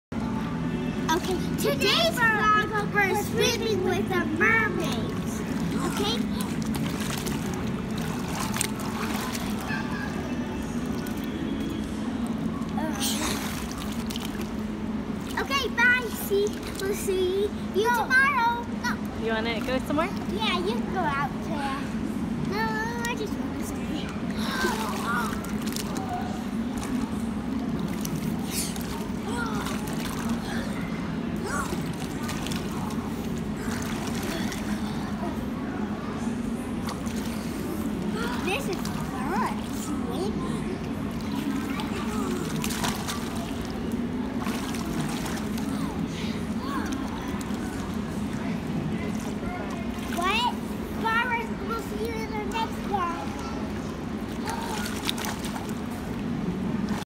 5year old twins take their mermaids swimming
The girls get to take their mermaids 🧜‍♀️  to the pool today! Fun times